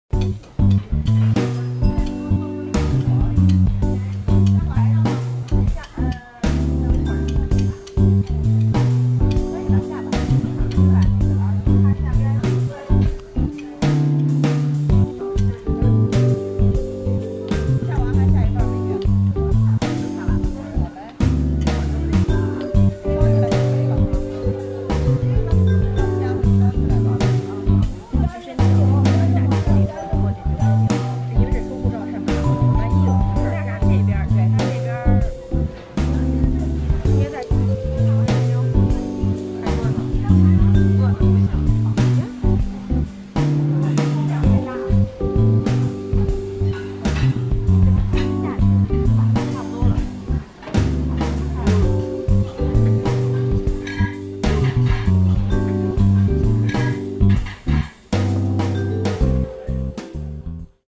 Traditional Peking hot pot
Jing Shuan hot pot restaurant in Chaoyang district, Beijing